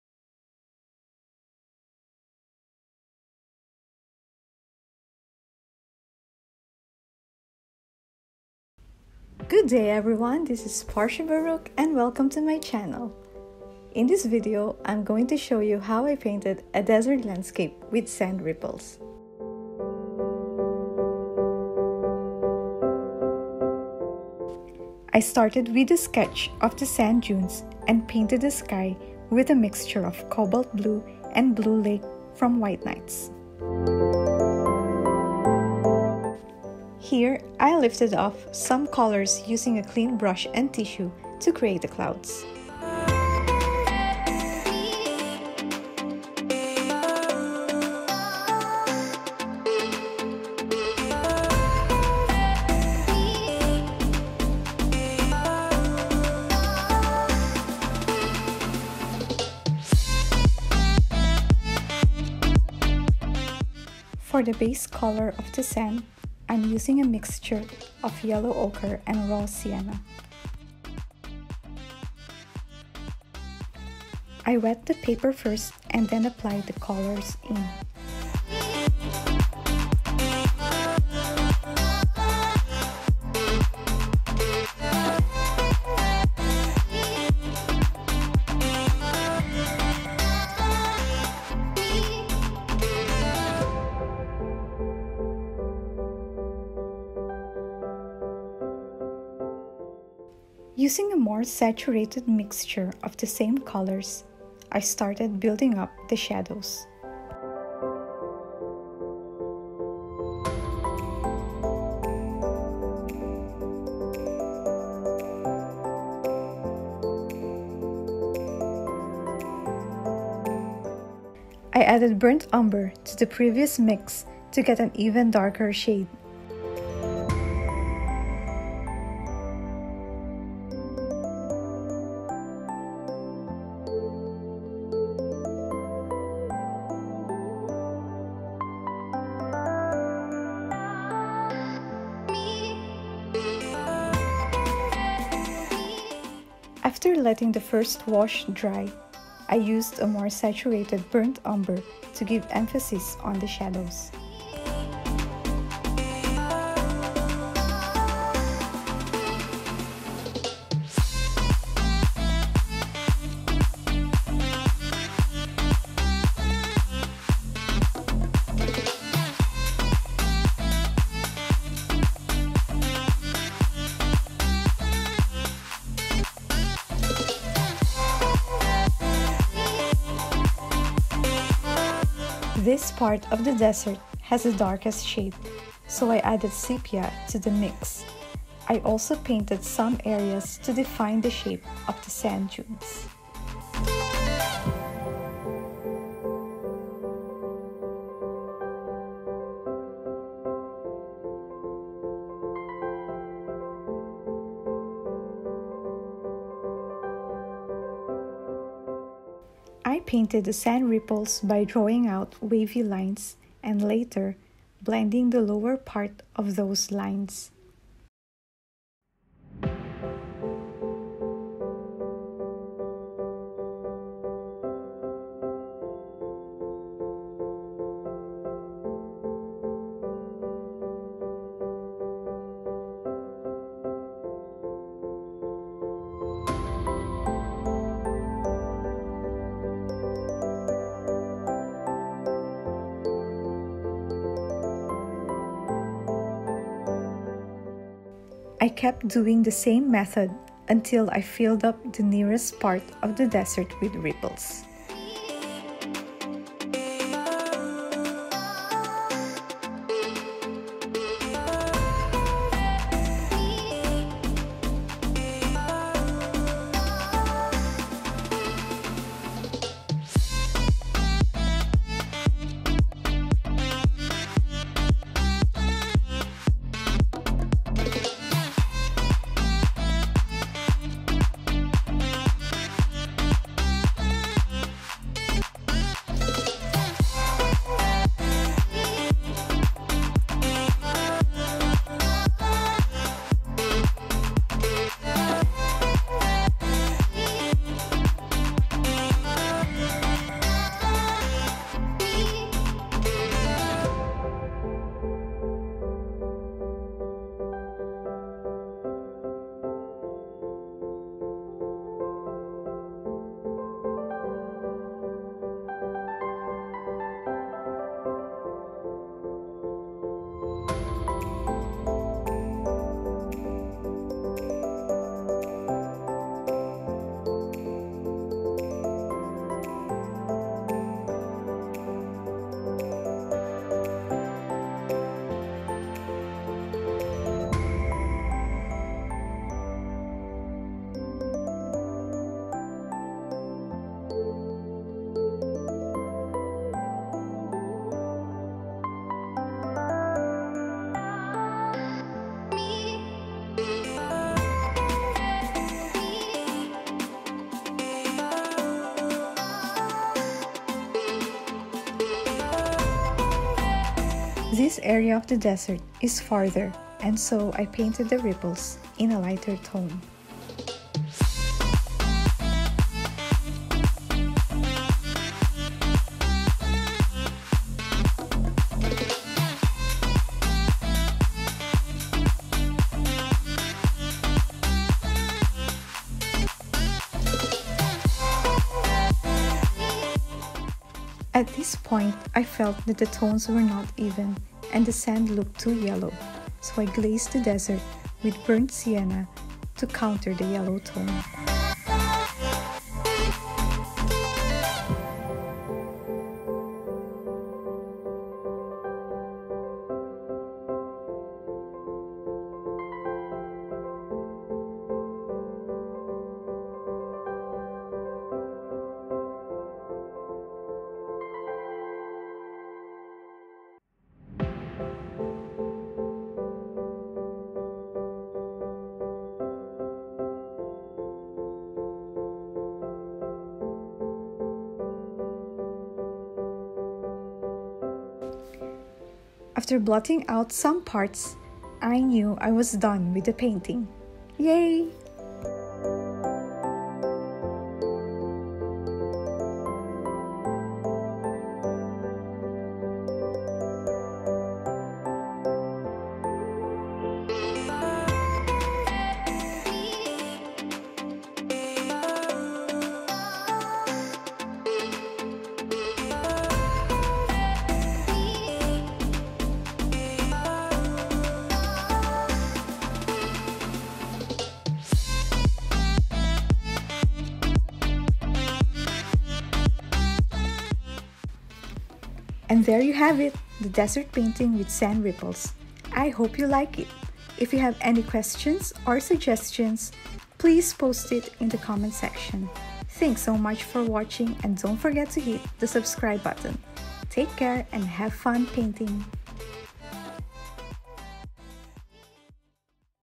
Painting Desert with Sand Ripples
Levelling up on my desert landscapes, here's a painting I did with sand ripples. Enjoy watching!

Let me know if you're painting it too! ❤️

🛍 SHOP UPDATE: This painting is now available as an art print!

Materials Used:
Escoda Ultimo 1/2" Oval Wash